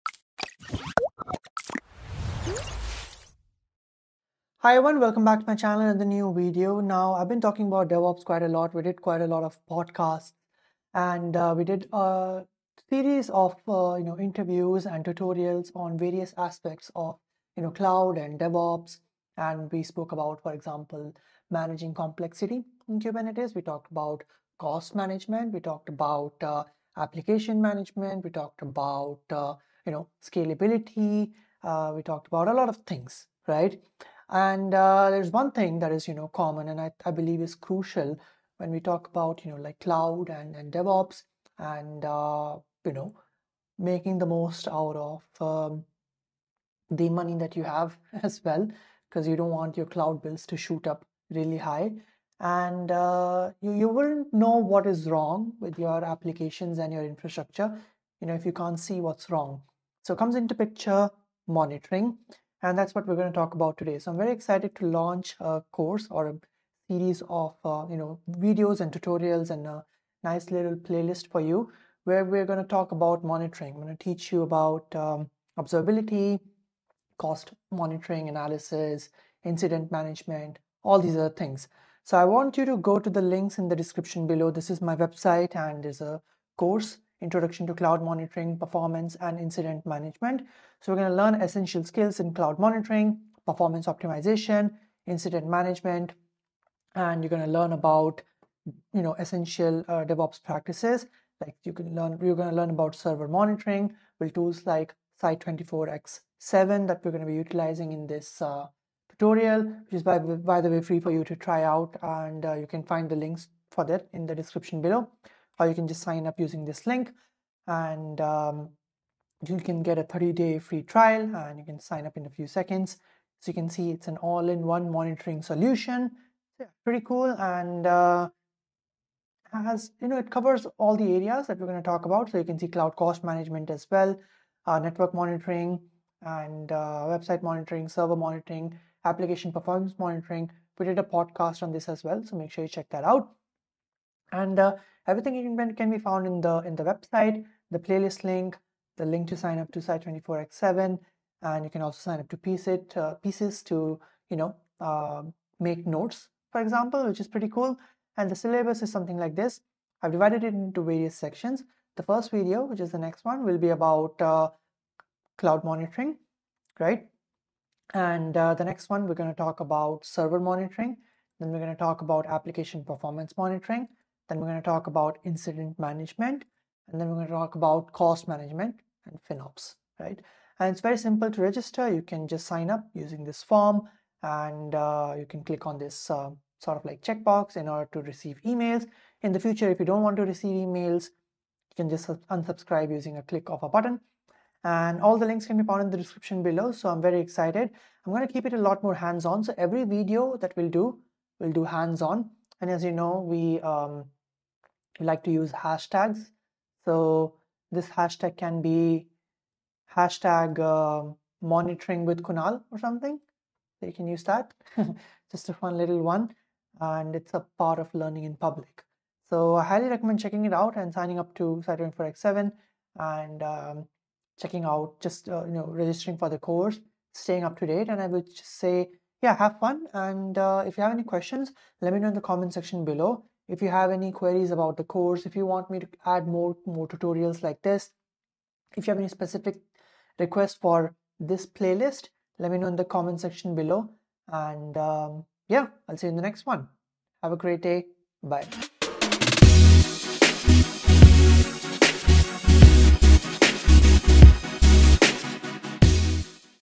[Course Announcement] Introduction to Cloud Monitoring, Performance, and Incident Management
In this course, you'll dive into the essentials of DevOps, exploring server monitoring with tools like Site24x7, optimizing application performance, mastering incident management protocols, and gaining insights into FinOps principles for efficient cloud cost management.